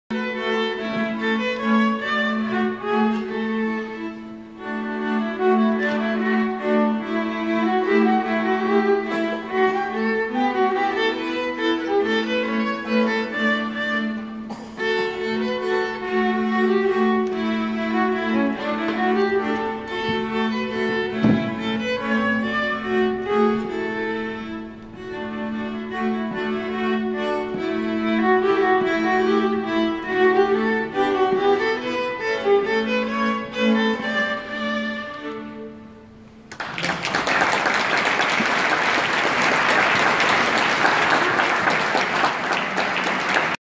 Lucija 2016-Mita in Jurij
Mita na nyckelharpi, švedskem ljudskem instrumentu. Spremlja jo soprog Jurij na violini.